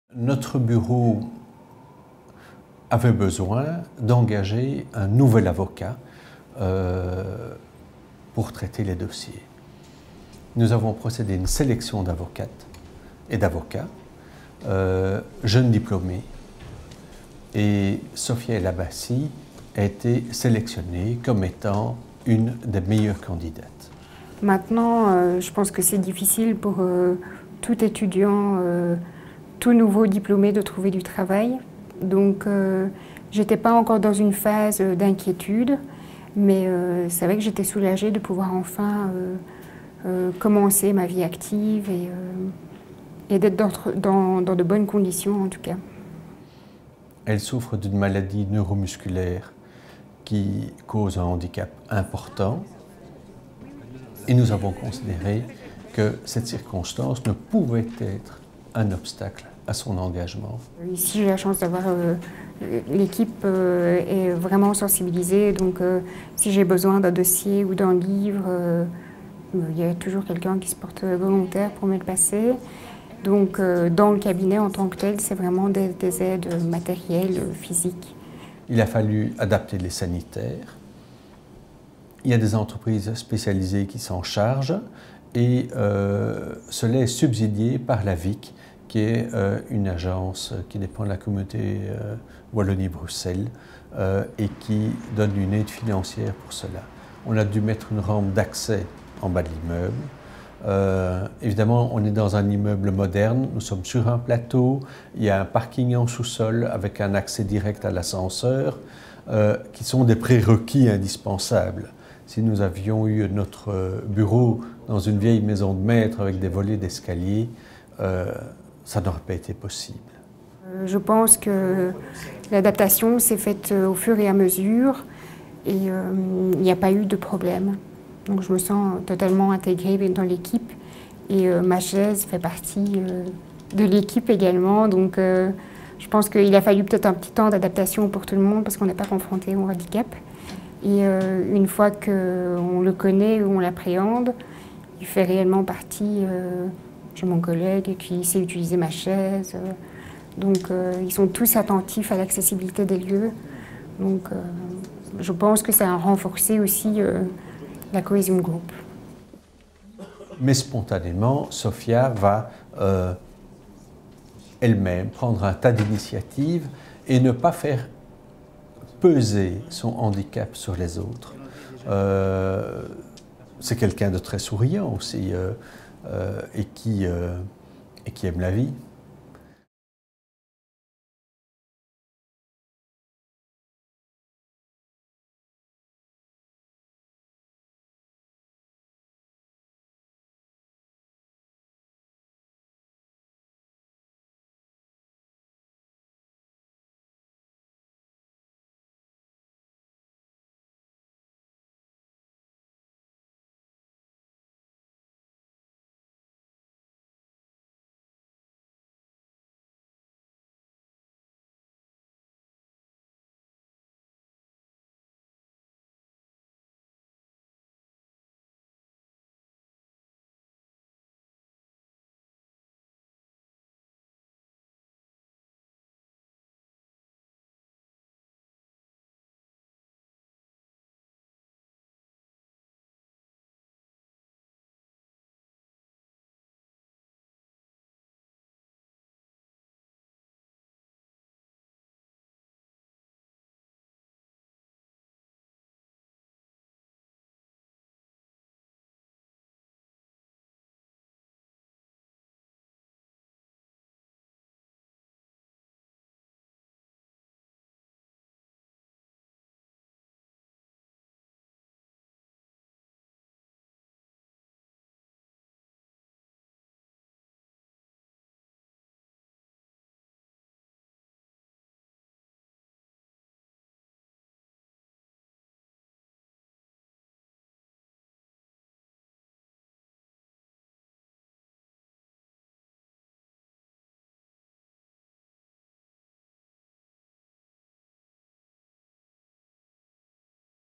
Coup de cœur du Jury du Prix CAP48 de l'Entreprise Citoyenne 2017 : JANSEN legal
Coup de cœur du Jury : JANSEN legal. Ce cabinet d'avocats compte 20% de son personnel à mobilité réduite suite à l’engagement de Sofia, jeune avocate. Le Jury a été séduit par la volonté d’intégration des personnes en situation de handicap de cette petite entreprise. Ce cabinet est la preuve concrète que l'engagement de personnes handicapées n'est pas réservé aux grandes entreprises ou aux organismes publics.